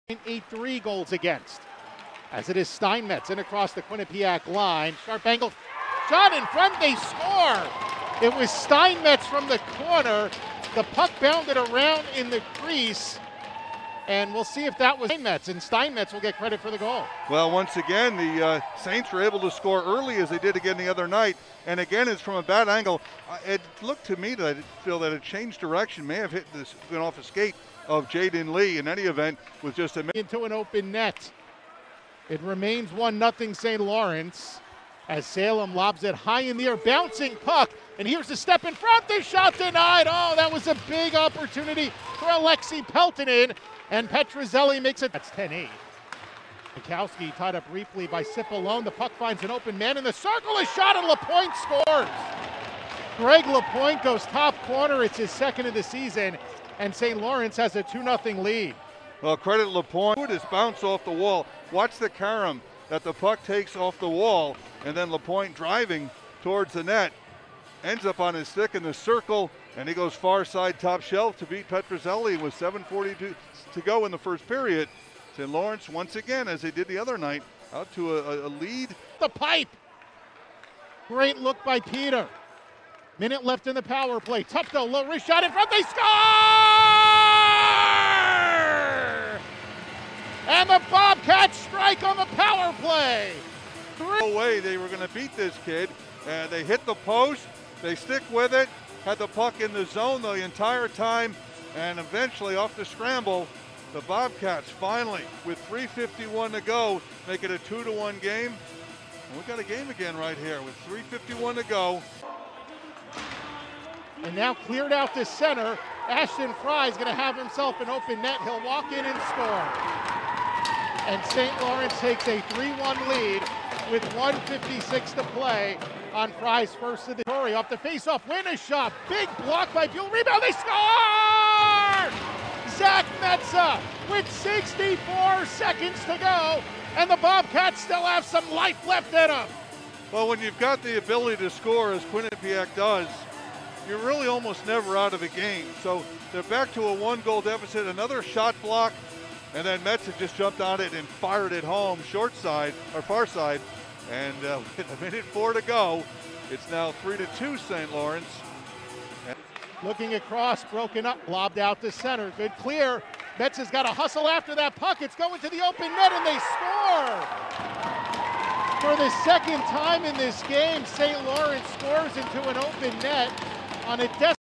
HIGHLIGHTS: St. Lawrence at Quinnipiac (01.03.21)
St. Lawrence picks up a big 4-2 win over Quinnipiac, using 37 saves from Emil Zetterquist and goals from four different skaters. It was the Saints first win in Hamden since 2014.